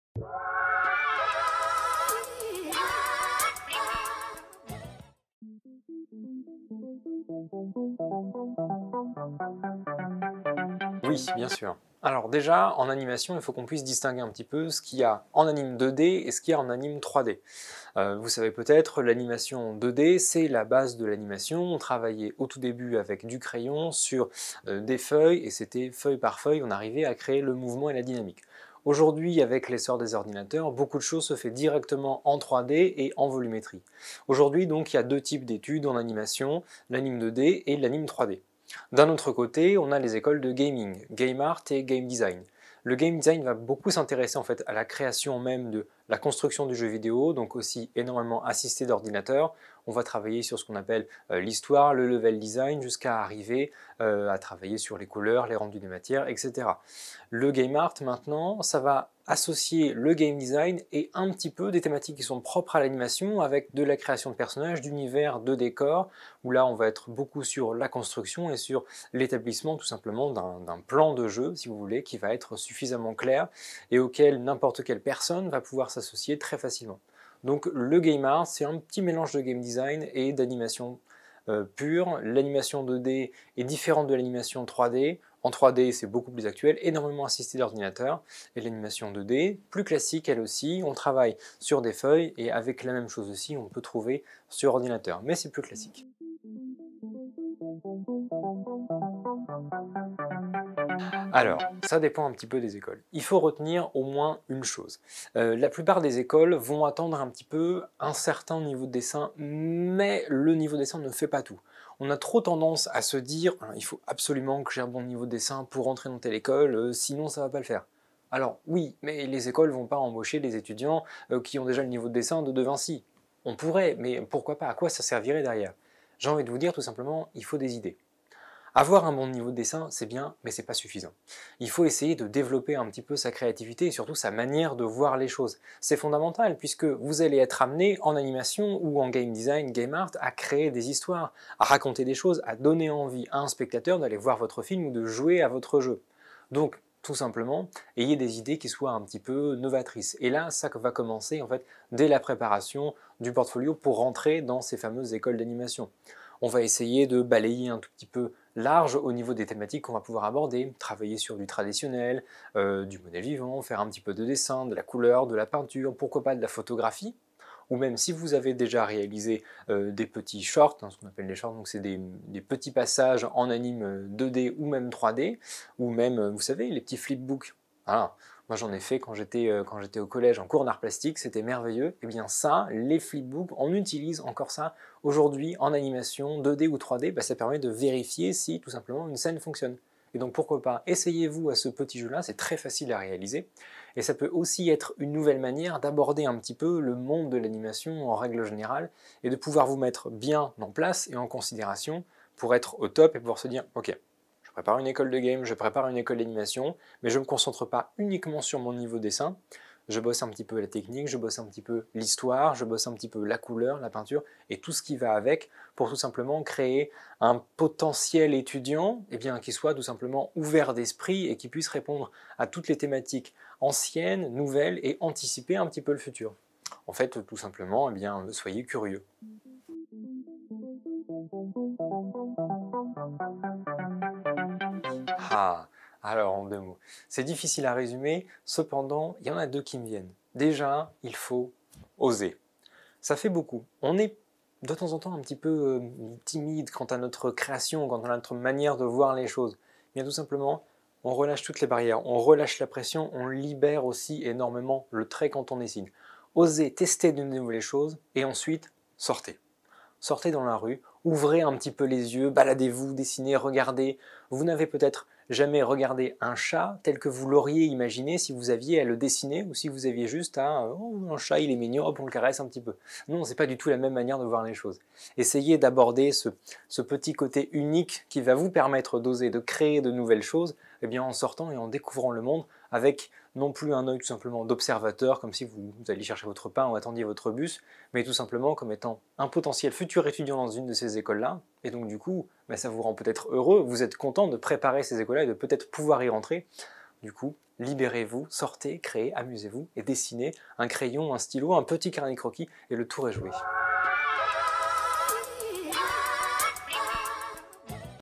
Conseils pour réussir son entrée en écoles d'animation
Professeur de gaming et d'animation chez Passart, Yohan Lacroix nous livre ses conseils et astuces pour réussir aisément le concours des écoles d'art en gaming et animation.